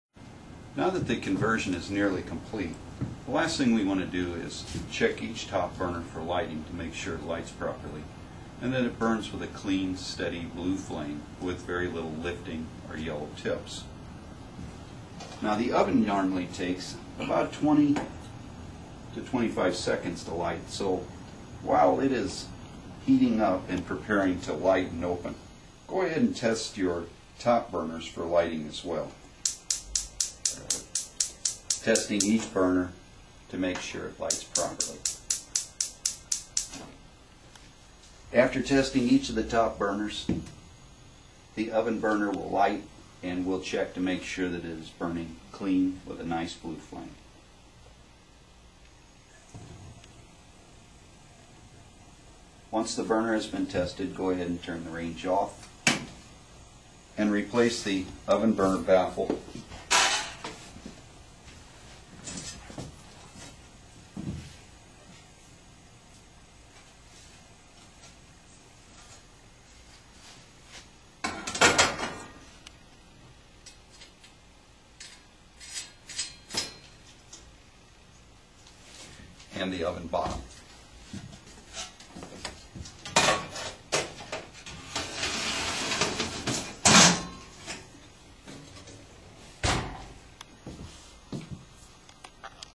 Step 4 Convert Sealed Burners
Step 4 - Converting your Premier Sealed Burner Range from Natural to LP - Checking Burners